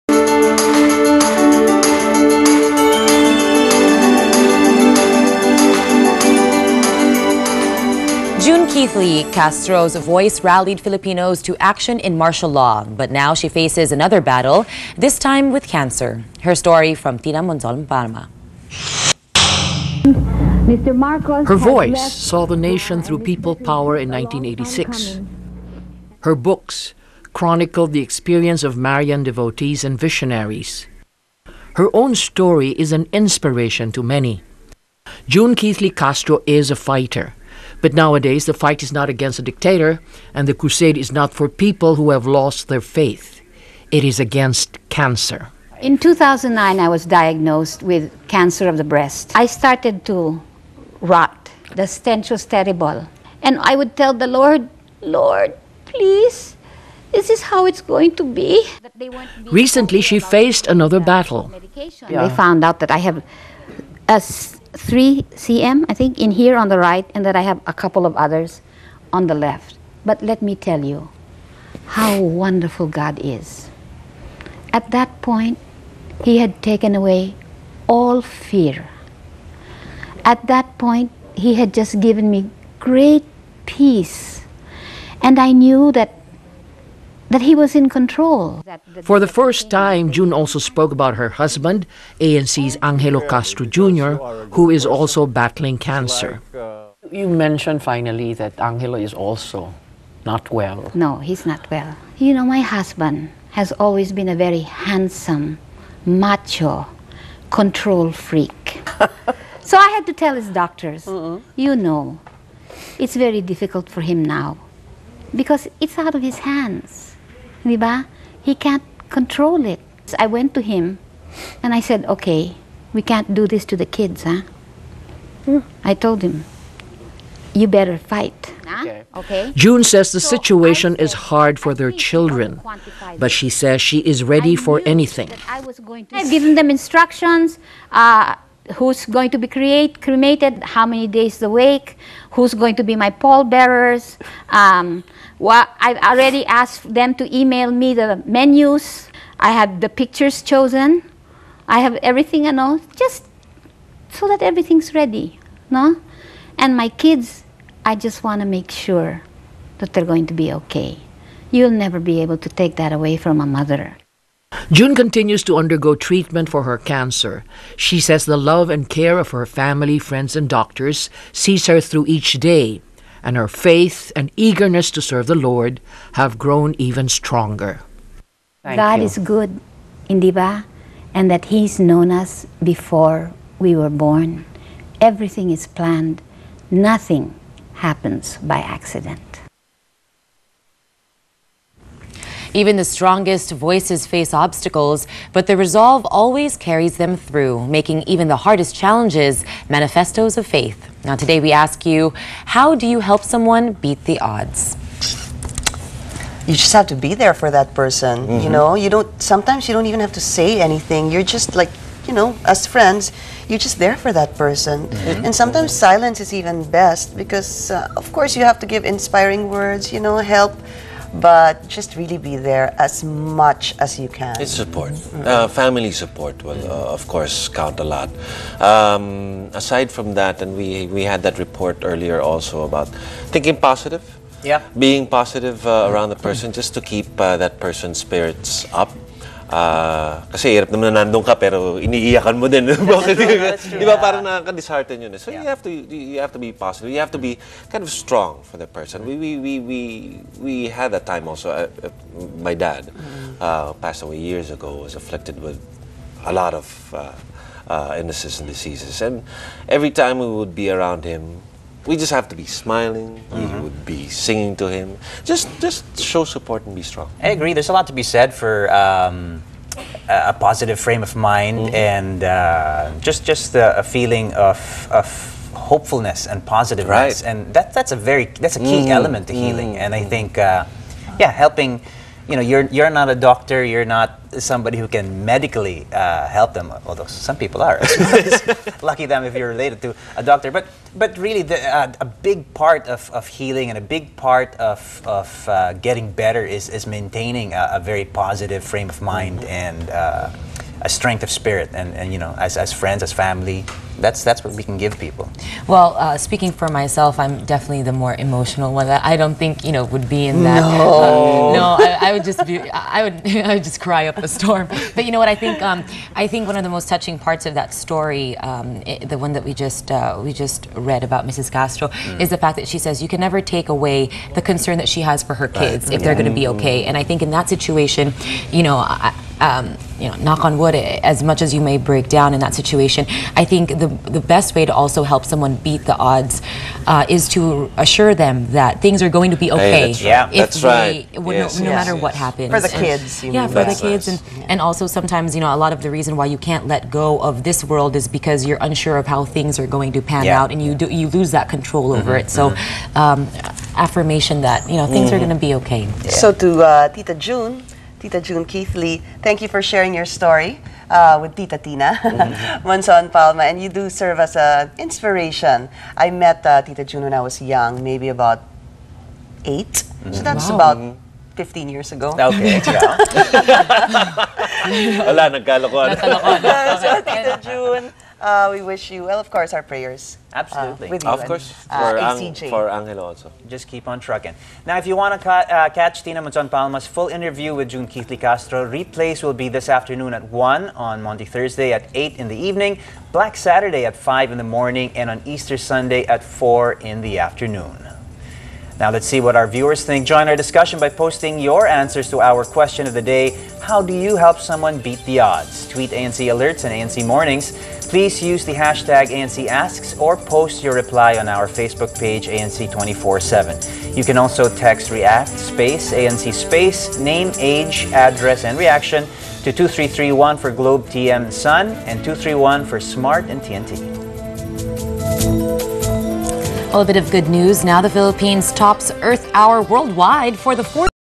Mornings Chat for QOTD
QUESTION OF THE DAY: How do you help someone beat the odds?